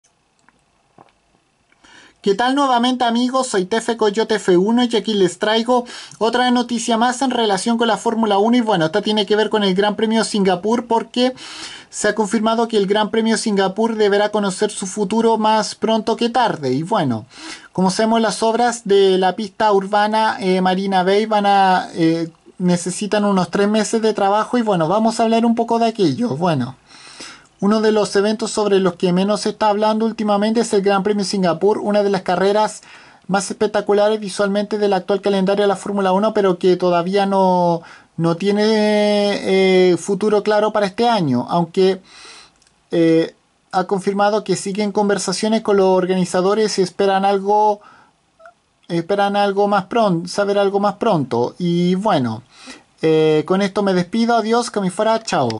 Se conocerá próximamente el futuro del GP de Singapur este 2020
Las obras del Marina Bay necesitan tres meses